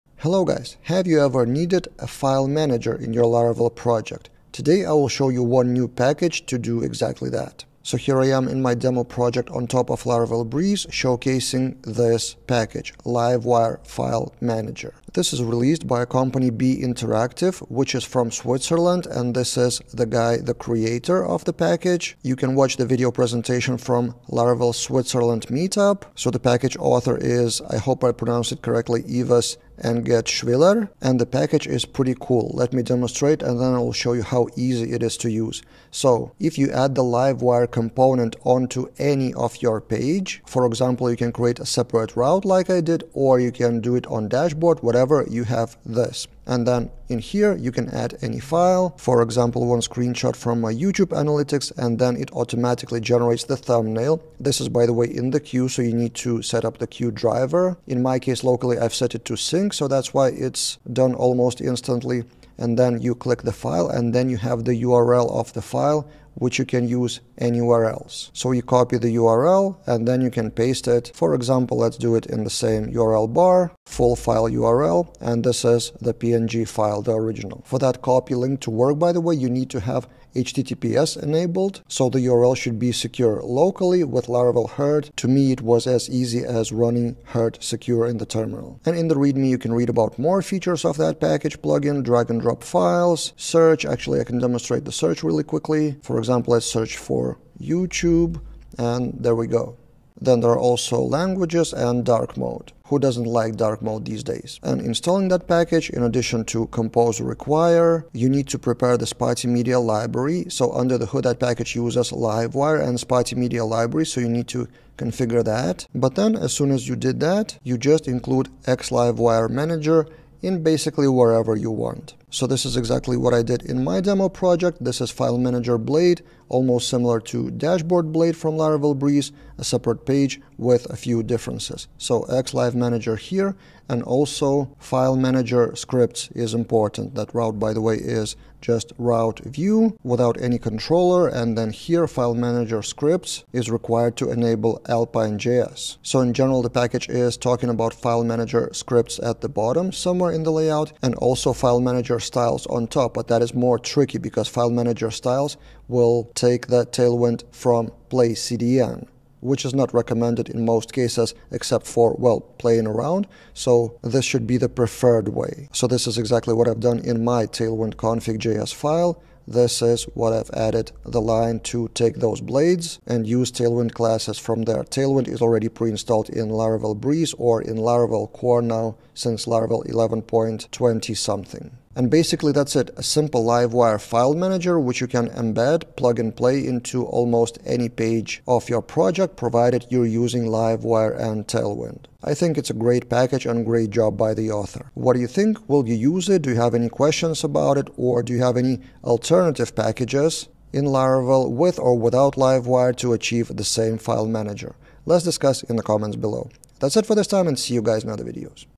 Laravel File Manager with Livewire: New Package
A very impressive easy-to-install package for file manager in your Laravel projects.